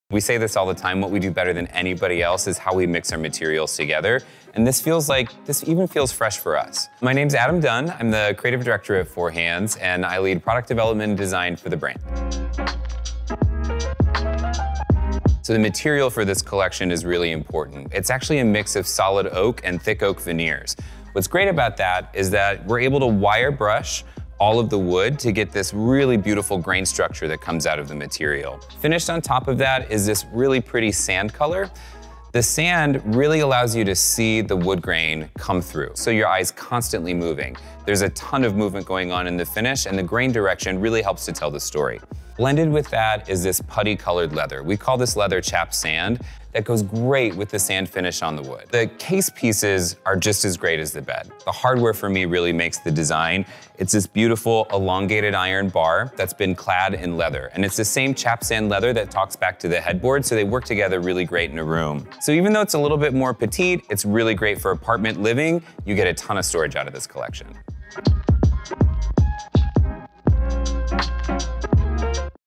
Behind the Design: Rosedale Bedroom Collection
Watch Four Hands Creative Director, Adam Dunn, talk about our new Rosedale Bedroom Collection -- a story of mixed materials.